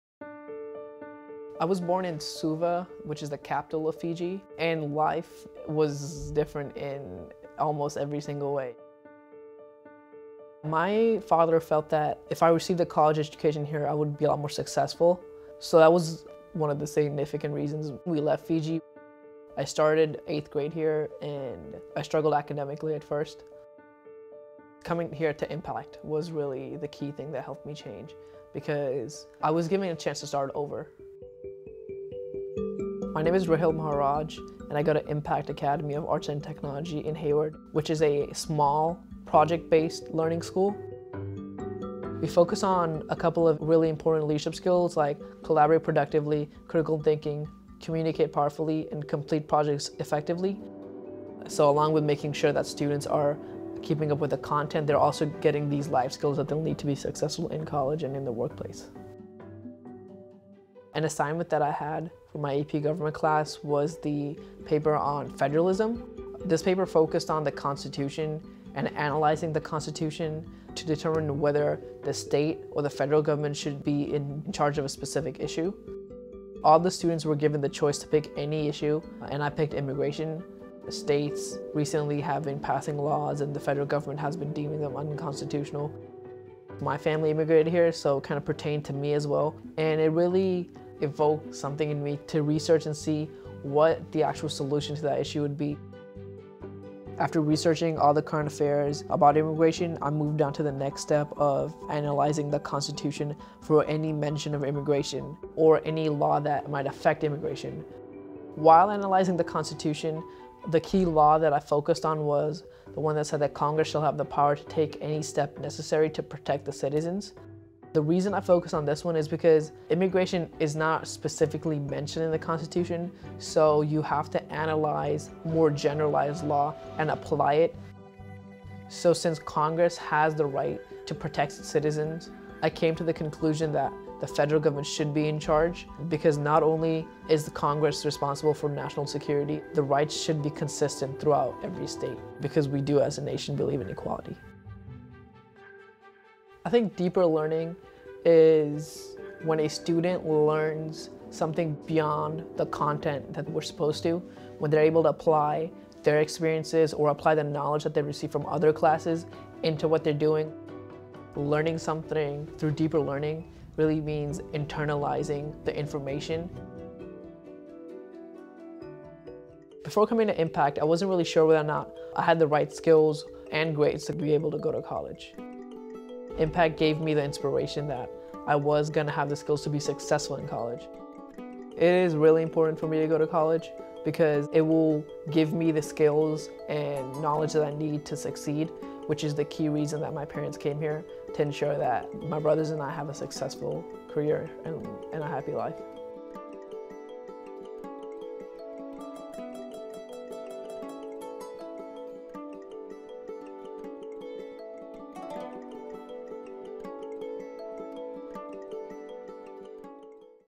Deeper Learning at Impact Academy
Rahil, a student at Impact Academy in Hayward, California, describes what the school's deeper learning approach -- a focus on critical thinking, problem solving, project based learning, and communicating effectively -- has meant for him and his family. Impact Academy is part of Envision Schools, a group of schools dedicated to preparing students to succeed in a changing world.